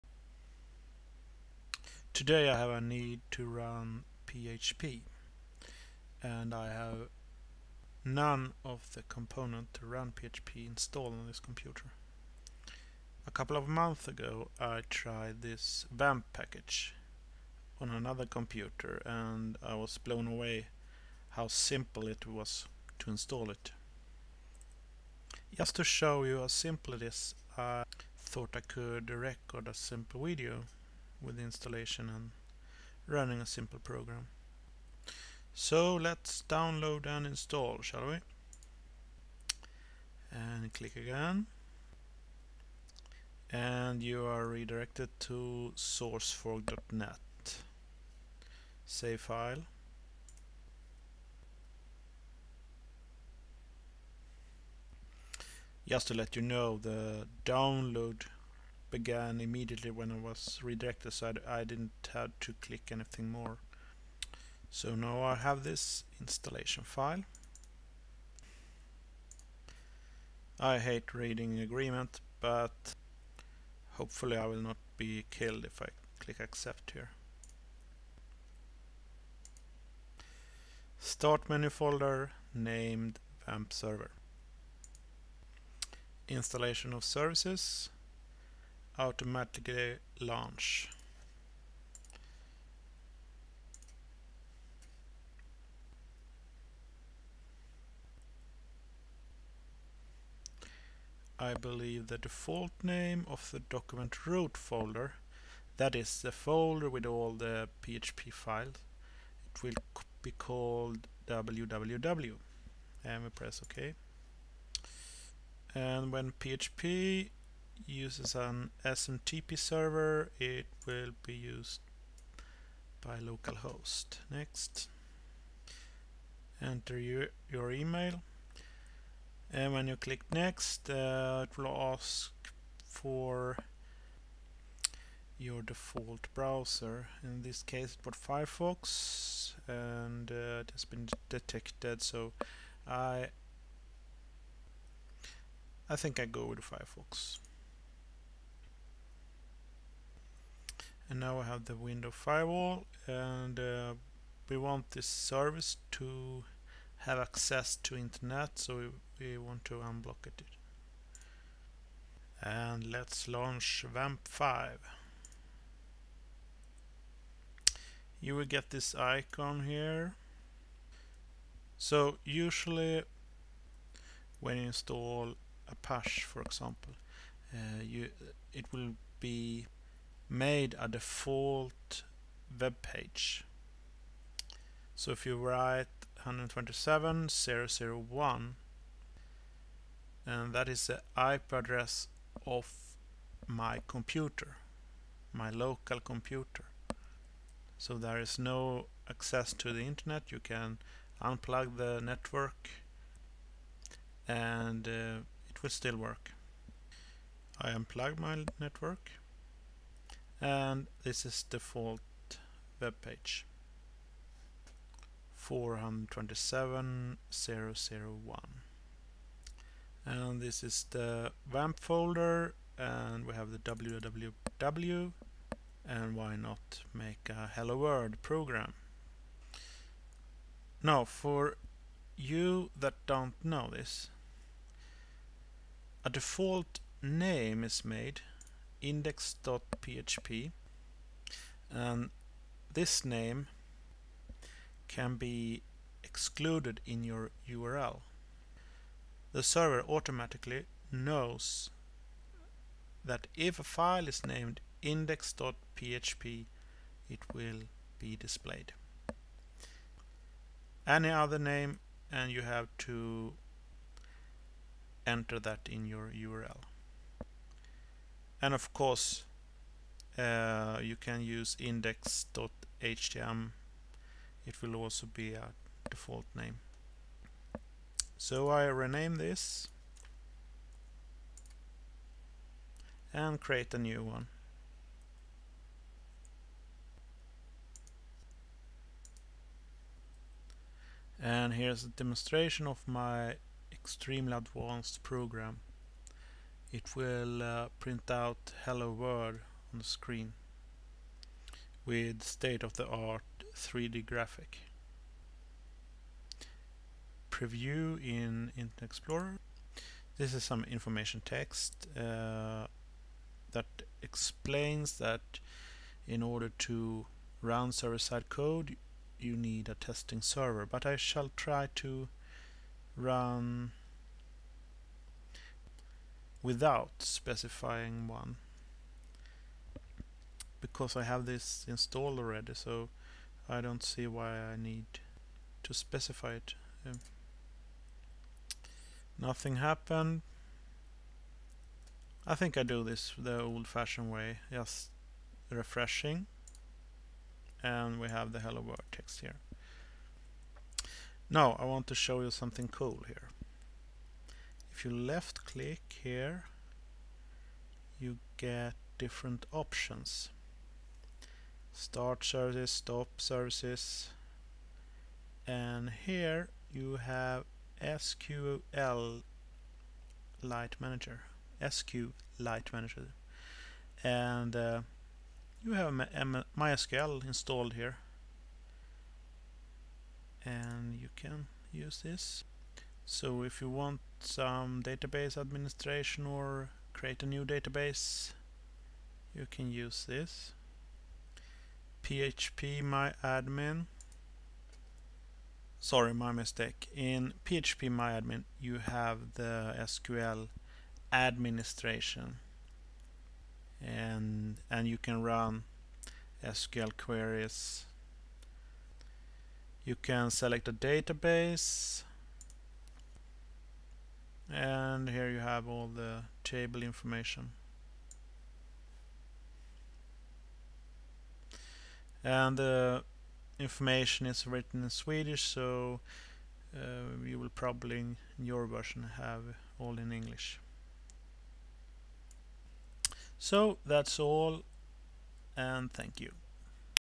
Installing WAMP
This video is about how to install php with mysql and apache as simple as possible.